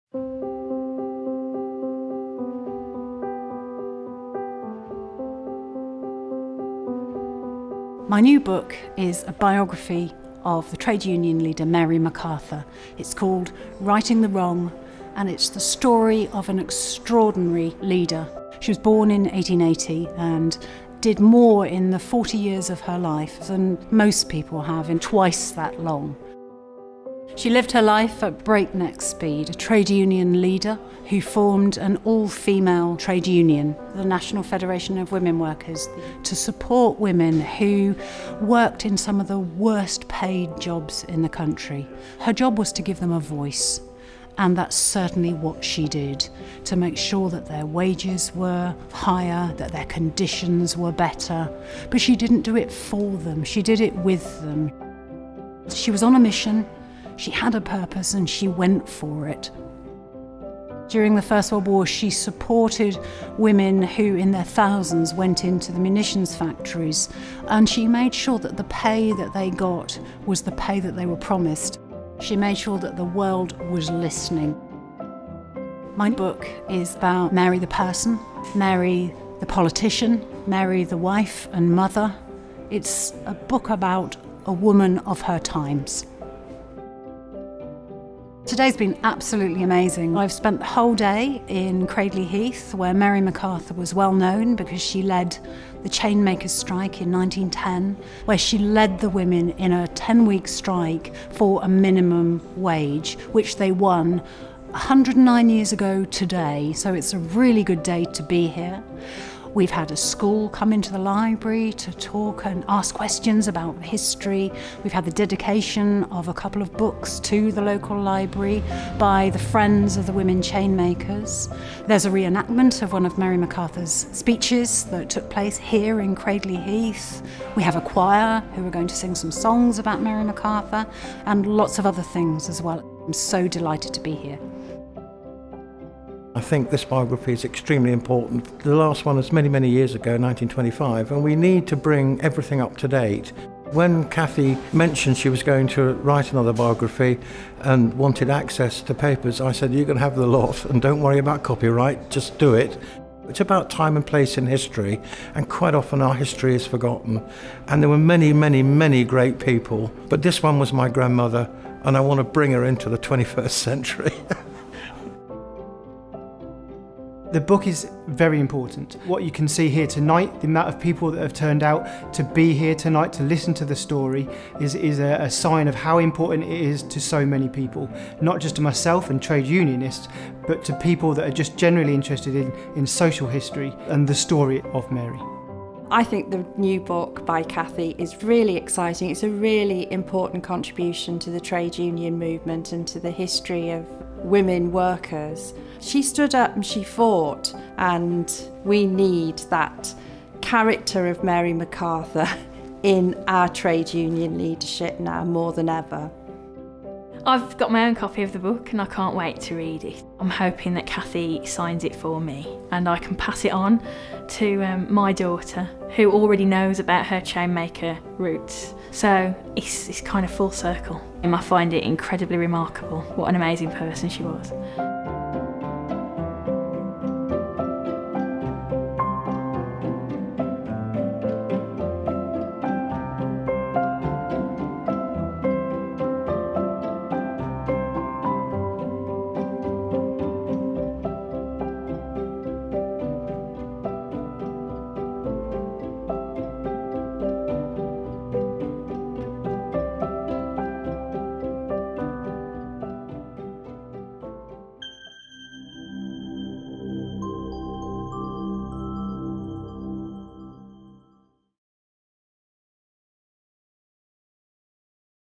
Righting the Wrong - Mary Macarthur 1880-1921: The Working Woman's Champion
In 1921 the brilliant and charismatic trade union leader, Mary
Macarthur, died aged 40. In her short life, her activism
and leadership had been responsible for raising awareness of
women’s poor working conditions and encouraging them to speak
out against injustice and inequality.

Mary Macarthur is perhaps best known for the prominent part
she played in the women chain makers’ strike in Cradley Heath,
Staffordshire in 1910. The dispute, which lasted two months, ended in success with the women receiving the country’s first minimum wage. It was a triumph but it was by no means an isolated event and Mary Macarthur, as leader of the country’s first all-female general trade union, the National Federation of Women Workers, travelled the length and breadth of the country making sure that women were strengthened by better pay and working
conditions and union membership.

This new account of her life seeks to understand what motivated this extraordinary individual and why she chose the path that she did, particularly at a time when it was still far from common for a middle -class woman to appear on public platforms. In other words, this is not just an account of Mary the union leader but of Mary the woman – of her travels and friendships, love and marriage, family and motherhood – explored within the context of her times.